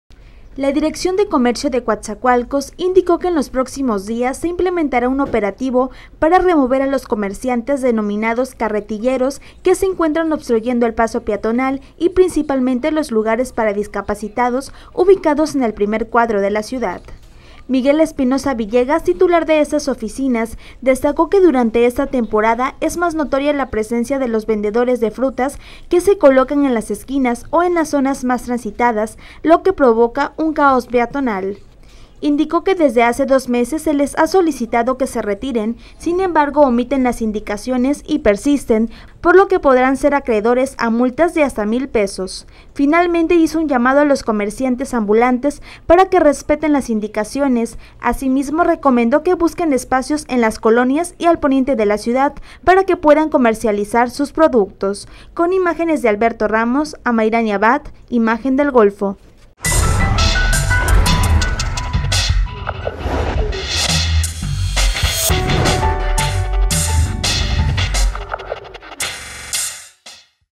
'Limpiarán' de ambulantes el centro de Coatzacoalcos
La Dirección de Comercio indicó que se implementará un operativo para remover a los comerciantes denominados 'carretilleros' que obstruyen el paso peatonal en el primer cuadro de la ciudad.